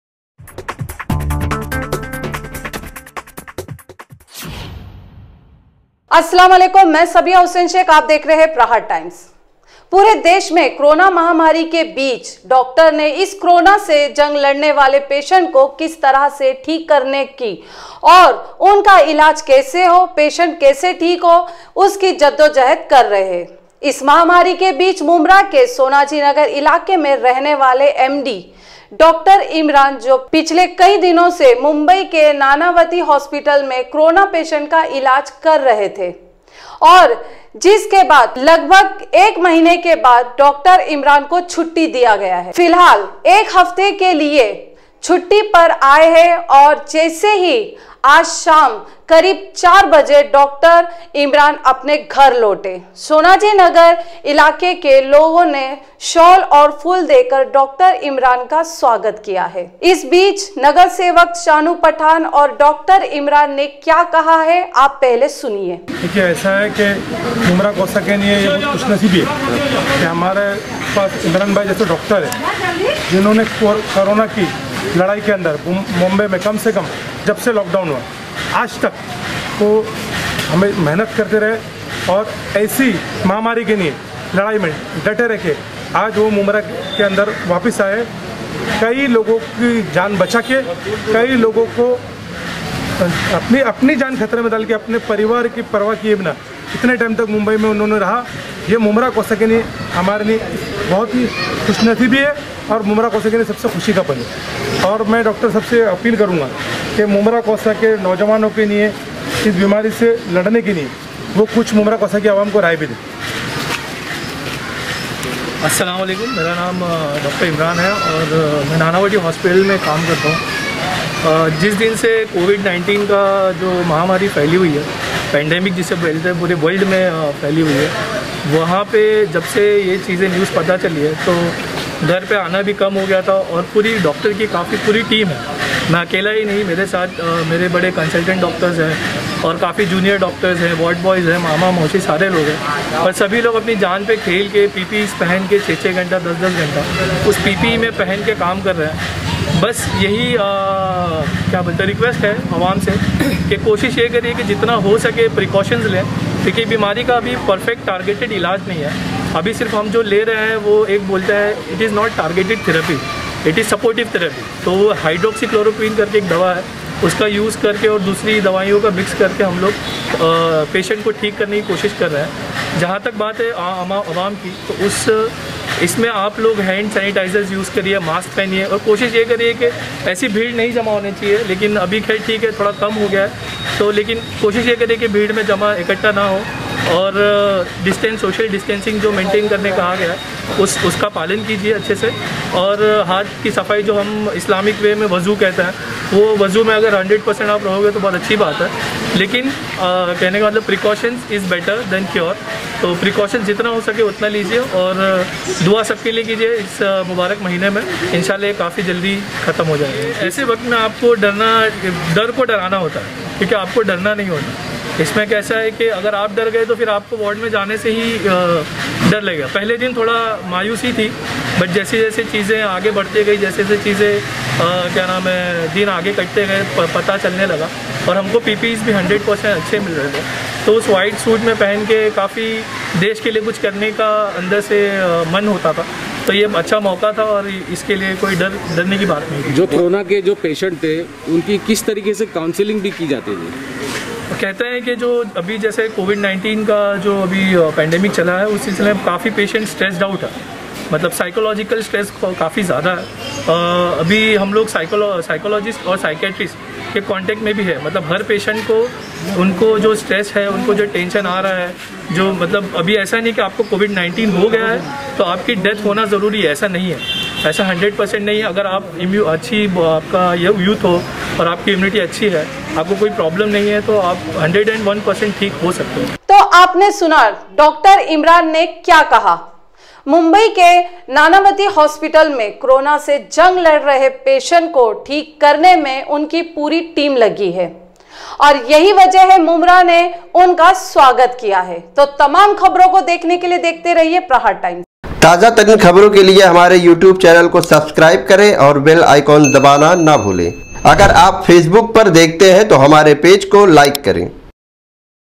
सोनाजी नगर में रहने वाले डाँ का स्वागत ,कोरोना पेसेंट का इलाज कर रहे थे इमरान||Aapka Prahar Times
सोनाजी नगर में रहने वाले डाँ का किया स्वागत
कोरोना पेसेंट का इलाज कर रहे थे इमरान